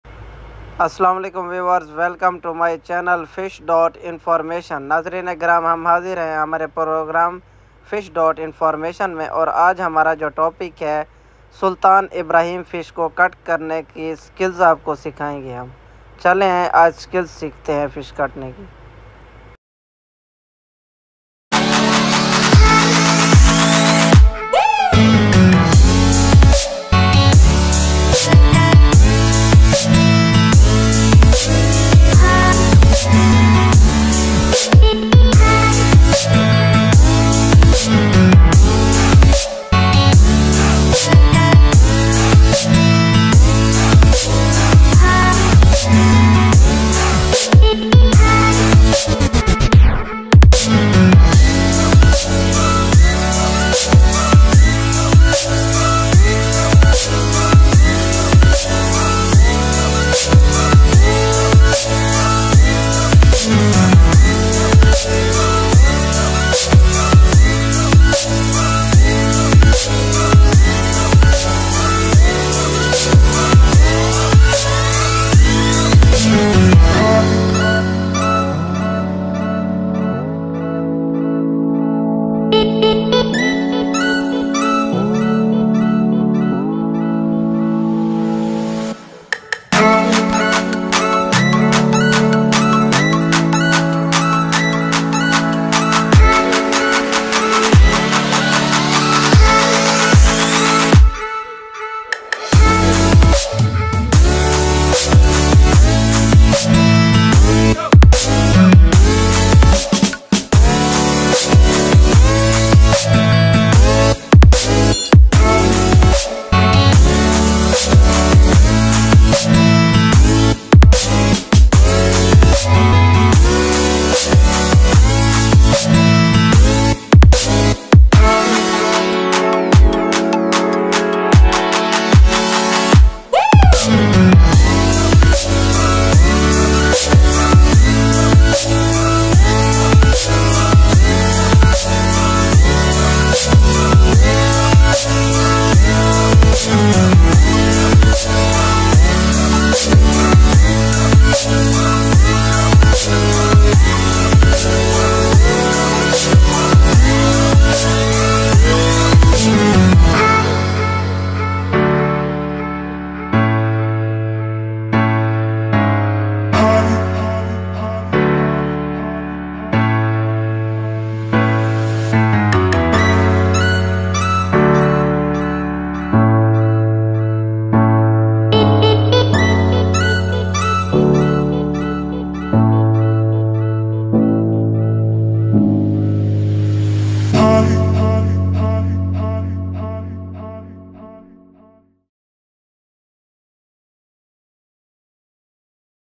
How to clean & cutting kilimeen fish| how to cutting kelli meen fish| Pink Perch fish cutting
Kilimeen people called out different names some people called kilimeen,Kelli meen,pink perch, red snapper small and റോമിയോ കിളി

This video is this fish cutting skills how to cutting kilimeen fish,pink perch cutting skills Amazing fish cutting skills, how to cutting kilimeen at home easy